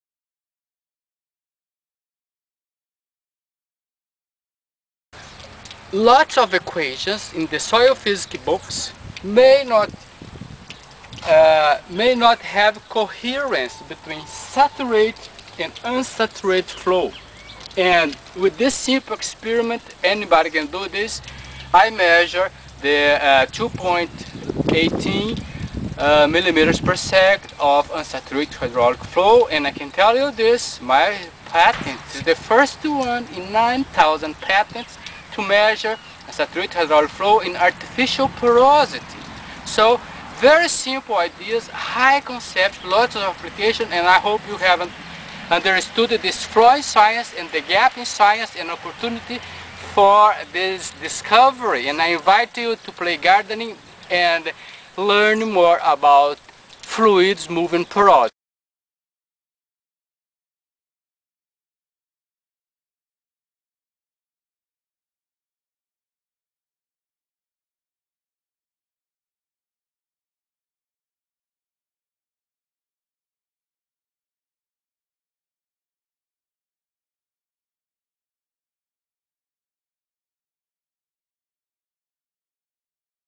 Tubarc 034 Flaw in Science I
There is a huge gap in science and technology in Hydrodynamics related to fluids moving on porosity offering new opportunities for thousands of new patents in the US.